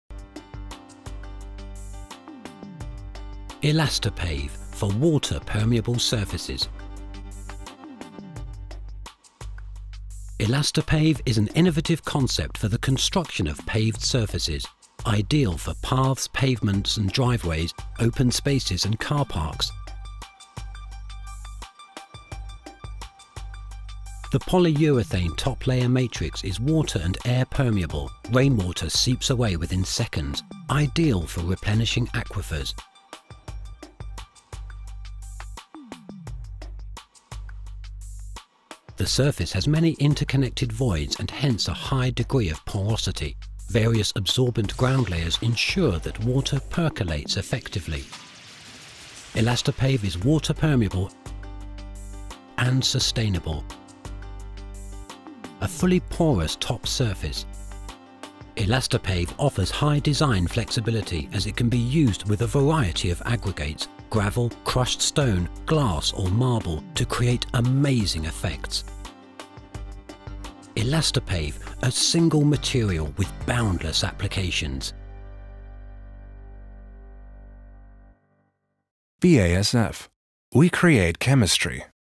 Elastopave® for water-permeable surfaces
Elastopave is an innovative concept for the construction of paved surfaces. Ideal for paths, pavements and driveways, open spaces and car parks. The new system offers numerous benefits in design, longevity and application diversity. Elastopave is an intelligent mix of selected stones with polyurethane. The surface has many interconnected voids and hence a high degree of porosity so rainwater seeps away within seconds – ideal for replenishing aquifers and a sustainable rainwater management. Elastopave is a single material for boundless applications.